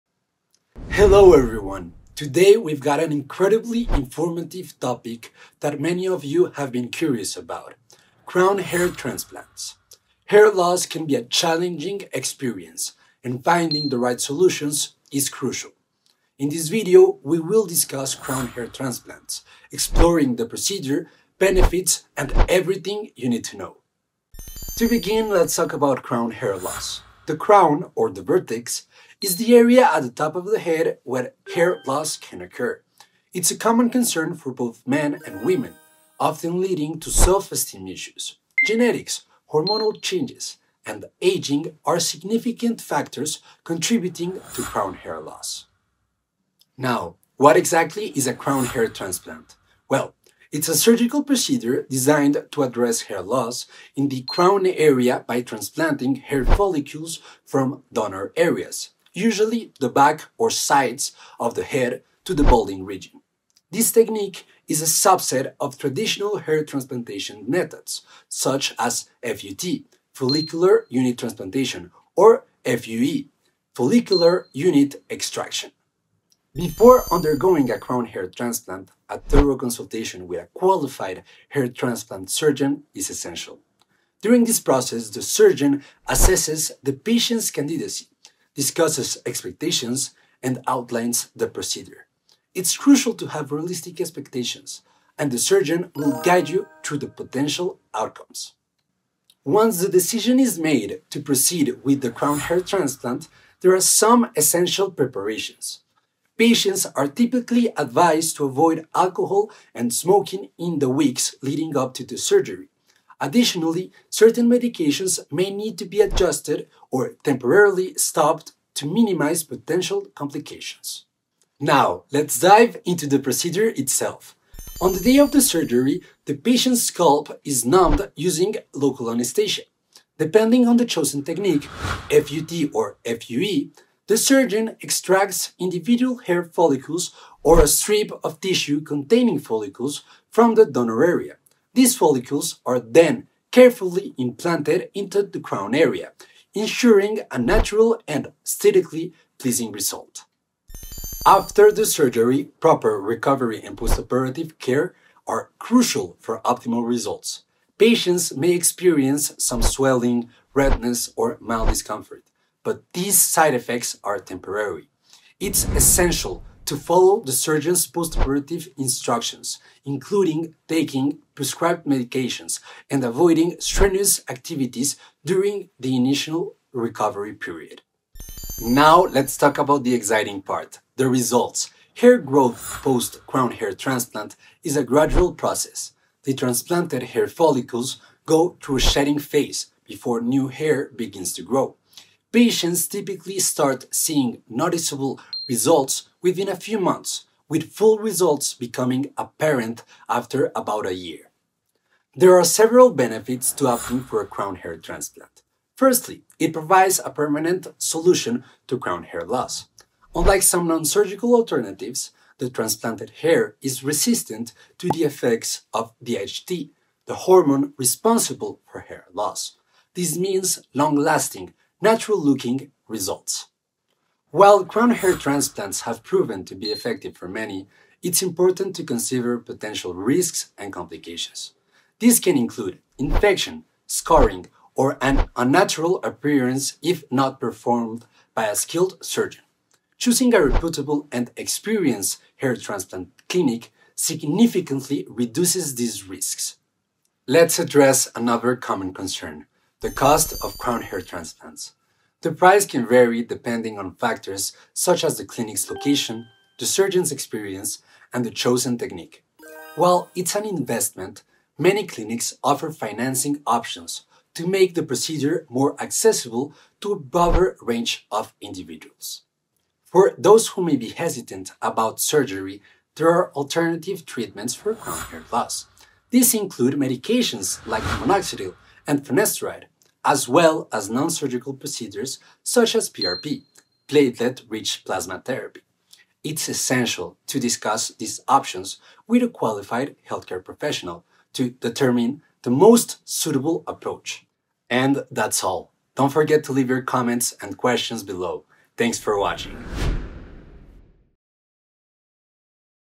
Crown Hair Transplant ✅
Hair Transplant on The Crown: Is It a Good Idea?

Also known as the vertex, the crown is the highest point of your scalp. Hair that grows in this area often follows a circular pattern which is called a hair whorl.
Balding at the ‘hair crown’ is a common experience that a lot of people go through at some point in their lives, especially those with a family history of hair loss.

For the professionals, the crown is a complex area.

Crown hair loss restoration is an effective way to address hair loss and thinning on top of the head. It can help patients feel confident again and look their best.

After a crown procedure, after one year, most of the transplanted hair will have fully matured although sometimes it may take up to 18 months.

TIMESTAMPS:
0:00 Intro
0:23 Crown
0:49 Crown hair trasplant
3:03 Post Crown hair trasplant
4:10 The cost